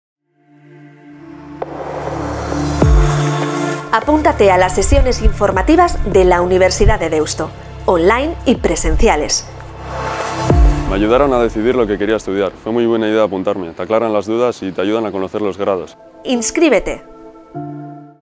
Aitor Goitia. Sesiones informativas y puertas abiertas. Universidad de Deusto
Aitor Goitia, alumno del grado en Ciencia de Datos e Inteligencia Artificial, recuerda la buena idea que tuvo al apuntarse a las sesiones informativas y las jornadas de puertas abiertas y cómo le ayudaron a decidir lo que quería estudiar.